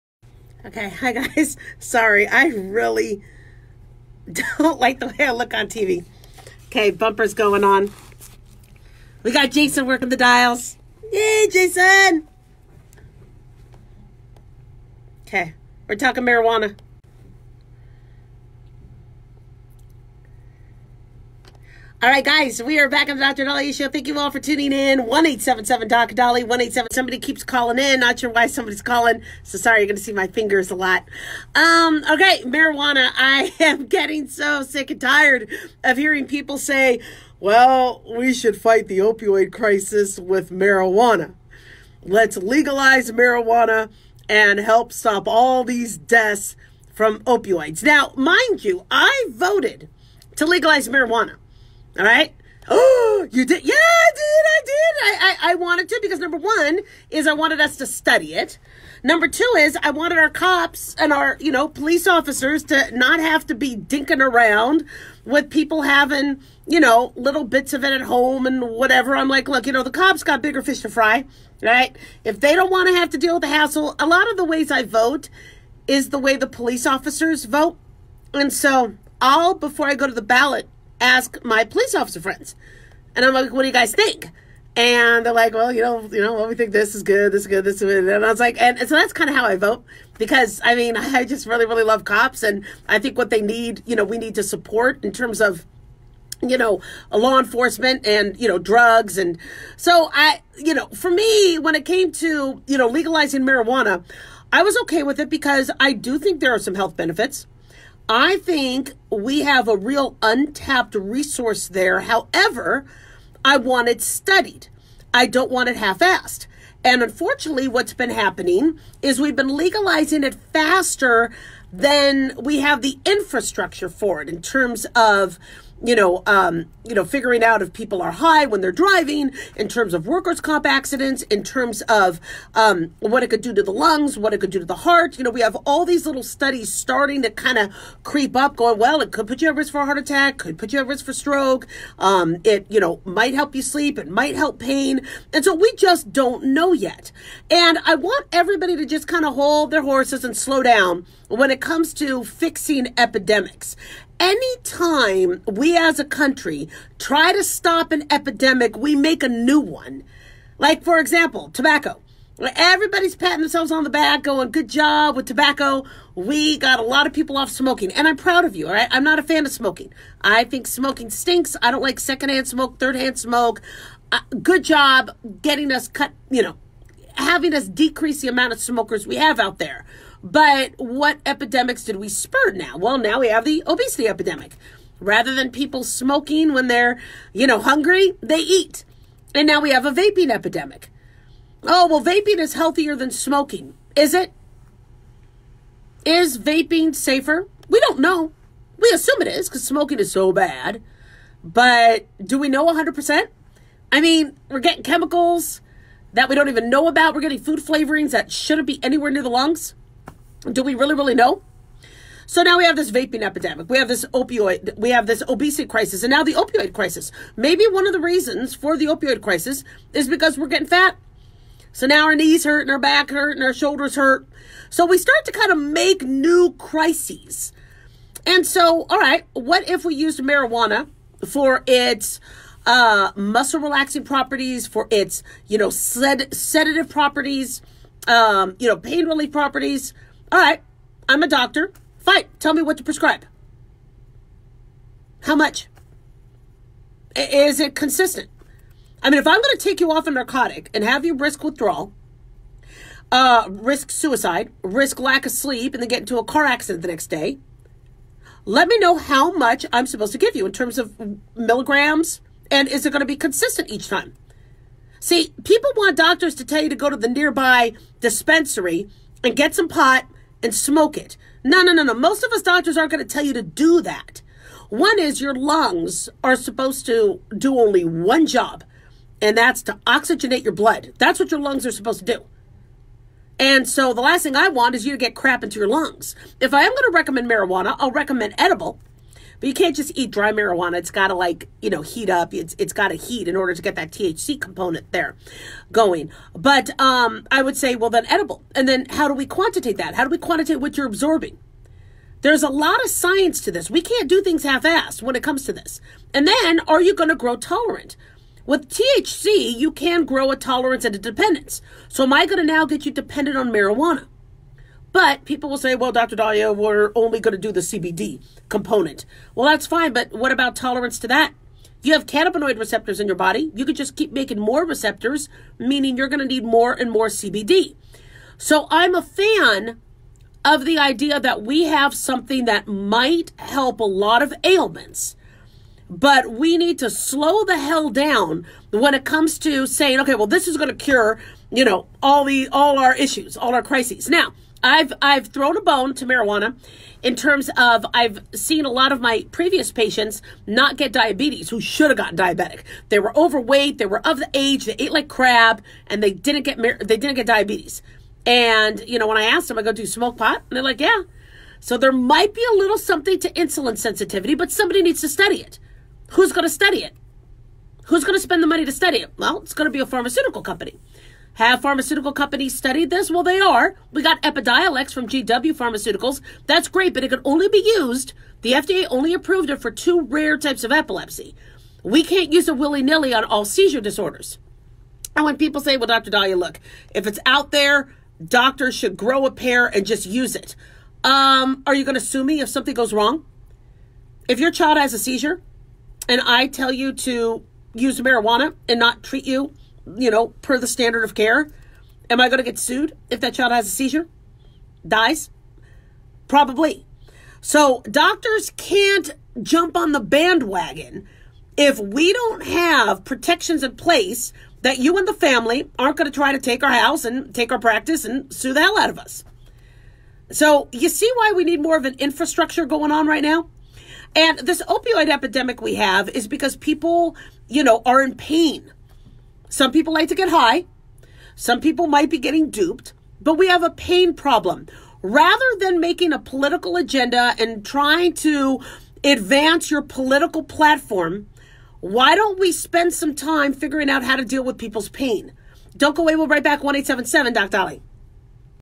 Should marijuana be used to fight the opioid epidemic....hold your horses...
Dr. Daliah explains how although marijuana can help relieve pain, patient may grow a tolerance to it